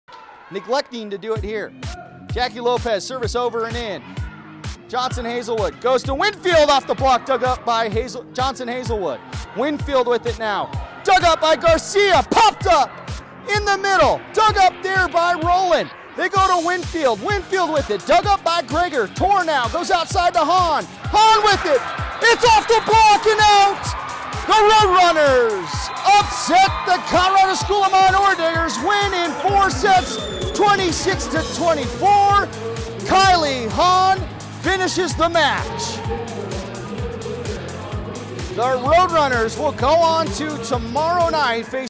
RMAC Top Play Week 11 Nominee - "Wrath of Haun"
Metro States junior Kylie Haun gets the match winning kill to upset the Colorado School of Mines in the semi finals of the RMAC volleyball tournament.